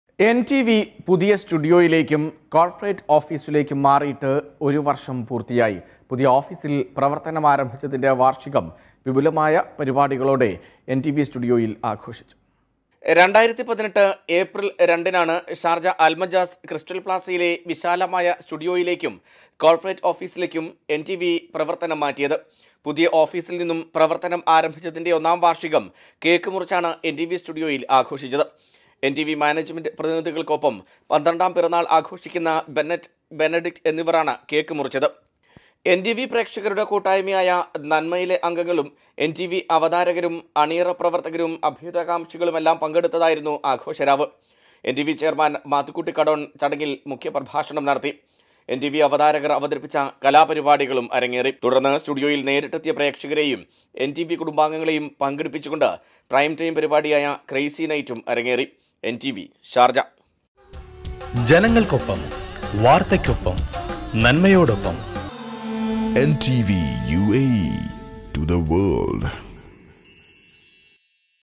എൻ ടിവി പുതിയ സ്റ്റുഡിയോയിലേക്കും കോര്‍പ്പറേറ്റ് ഓഫിസീലിക്കേും മാറിയിട്ട് ഒരു വര്‍ഷം
എൻ ടിവി പുതിയ സ്റ്റുഡിയോയിലേക്കും കോര്‍പ്പറേറ്റ് ഓഫിസീലിക്കേും മാറിയിട്ട് ഒരു വര്‍ഷം പൂര്‍ത്തിയായി.
പുതിയ ഓഫീസില്‍ പ്രവര്‍ത്തനം ആരംഭിച്ചതിന്റെ വാര്‍ഷികം വിപുലമായ ആഘോഷപരിപാടികളോടെ എന്‍ടിവി സ്റ്റുഡിയോയില്‍ നടന്നു.